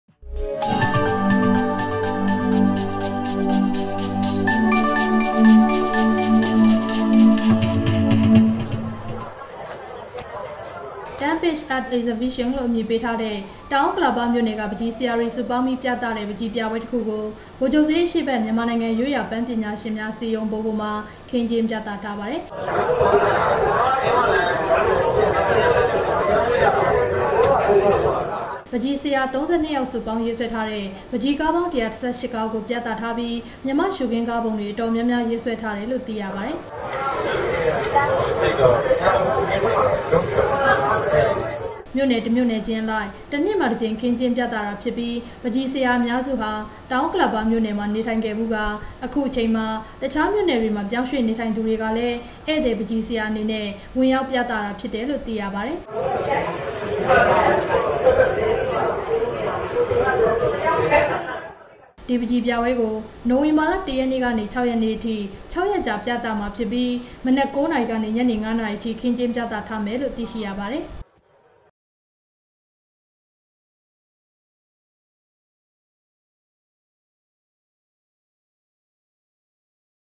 South Okkalapa 10 Page Art Exhibition
Tenth Page Art Exhibition လုိ႔ အမည္ေပးထားတဲ့ ေတာင္ဥကၠလာပၿမဳိ႕နယ္က ပန္းခ်ီဆရာ ၃၂ ေယာက္ရဲ႕ ပန္းခ်ီကားေပါင္း ၁၁၈ ကားကုိ ဗုိလ္ခ်ဳပ္ေစ်းအေရွ႕ဘက္ ျမန္မာႏုိင္ငံရုိးရာပန္းပညာရွင္မ်ားအစည္းအရုံး (ဗဟုိ) မွာ ႏုိ၀င္ဘာလ ၁ ရက္ေန႔ကေန ၆ ရက္ေန႔အထိ မနက္ ၉ နာရီကေန ညေန ၅ နာရီအထိ ခင္းက်င္းျပသထားပါတယ္။